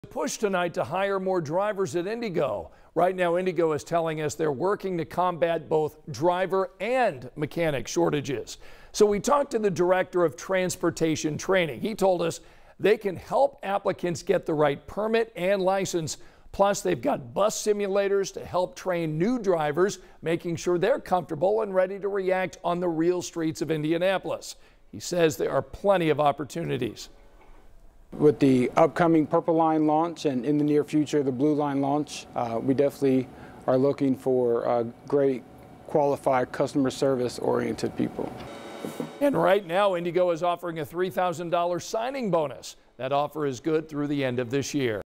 Addressing driver shortage at IndyGo
There's a push to hire more drivers and mechanics.